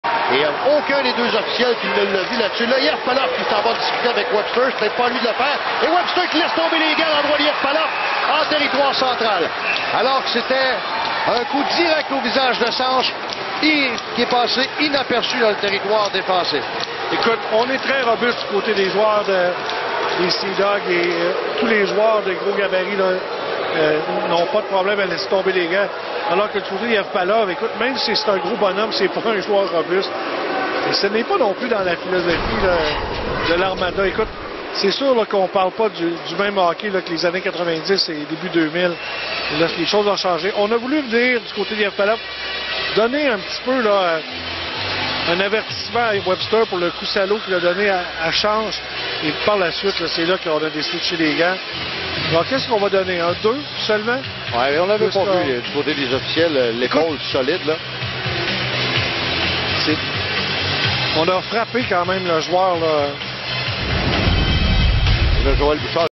Bailey Webster vs Nikita Jevpalovs Jan 31, 2015
Bailey Webster vs Nikita Jevpalovs from the Saint John Sea Dogs at Blainville-Boisbriand Armada game on Jan 31, 2015.